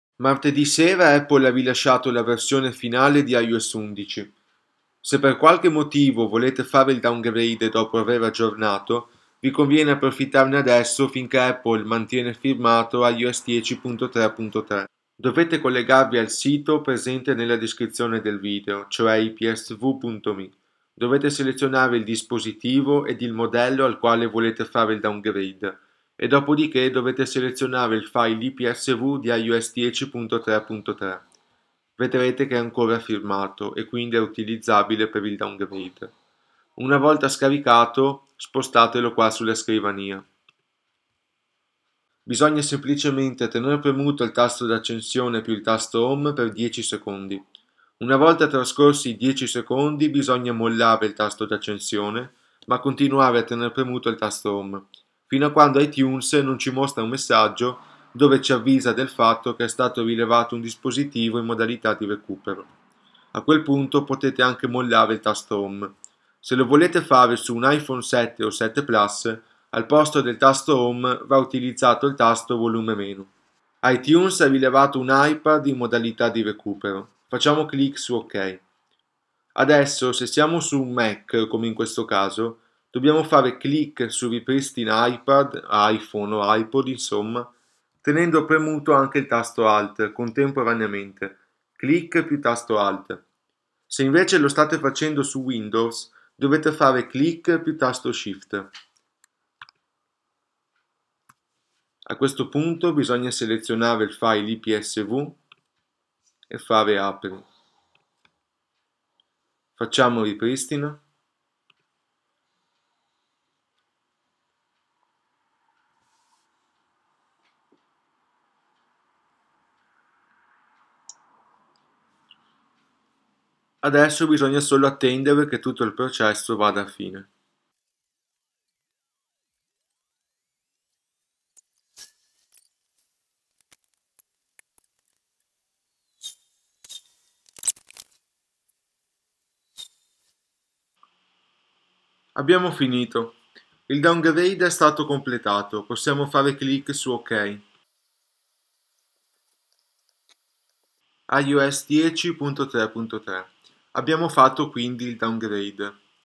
Downgrade iOS 11 a iOS 10.3.3
Vediamo come fare il downgrade da iOS 11 ad iOS 10.3.3 finchè Apple continua a firmarlo !